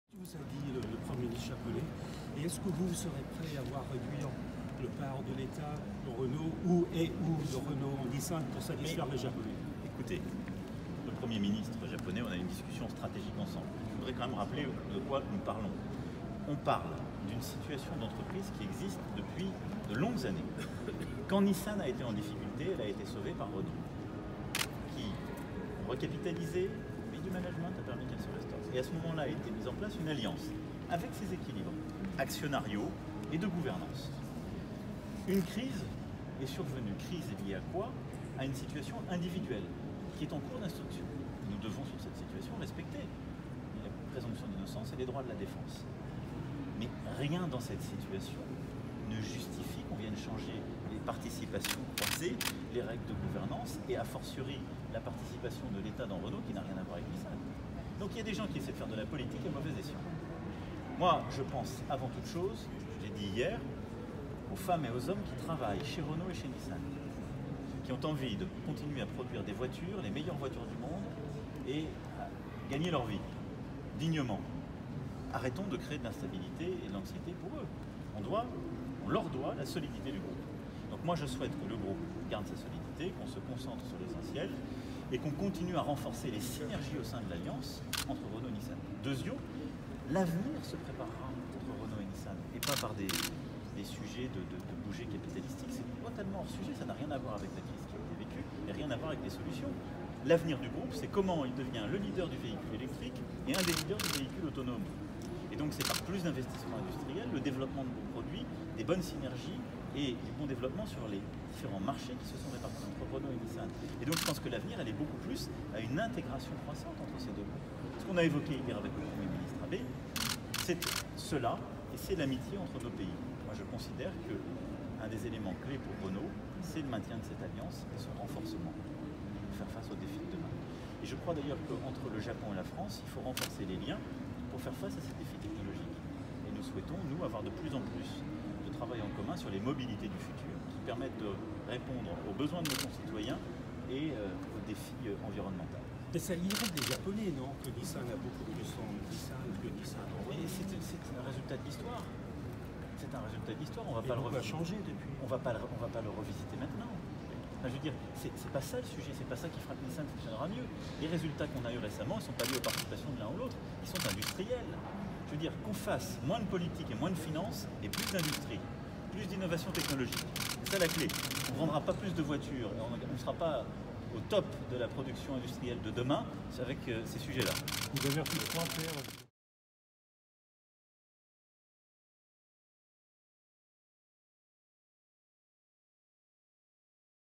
Macron : « L’avenir est à une intégration croissante entre Renault et Nissan »
Depuis le Japon, Emmanuel Macron s'est exprimé sur la situation tendue au sein de l'alliance entre Renault et Nissan.

Le président de la République considère que « rien ne justifie que l'on vienne changer les participations croisées, [ou] les règles de gouvernance. »